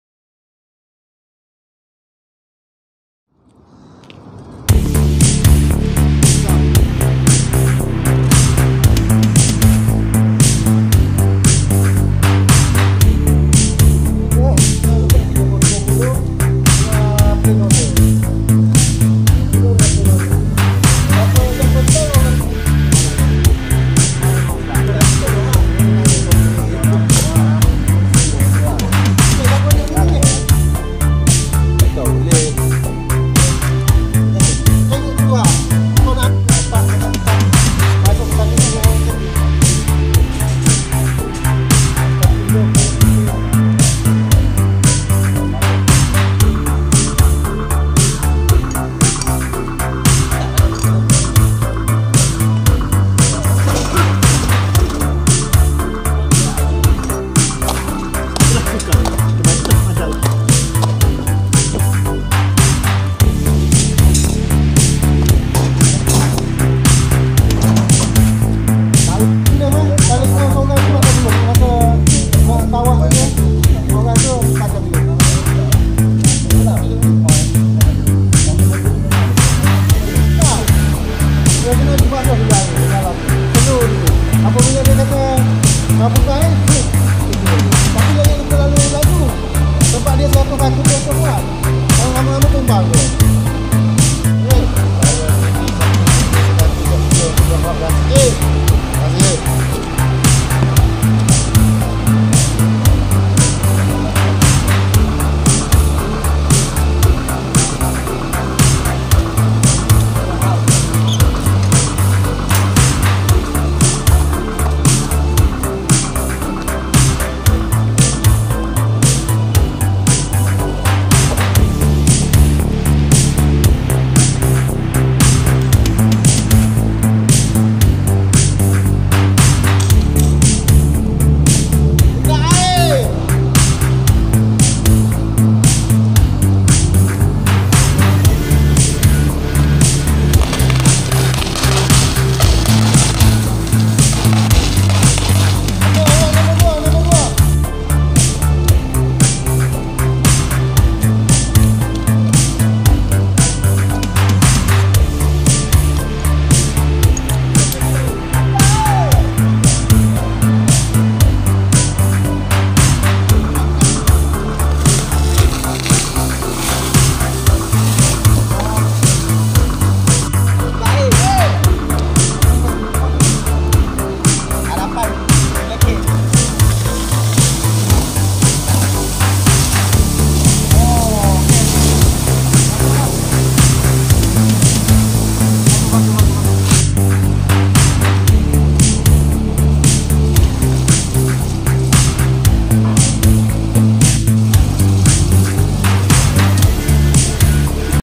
JTH BBP Puchong Kawad Operasi Basah
JTH Pasukan C BBP Puchong
Kawad Operasi Basah Membentang dan menggulung hose 100 kaki..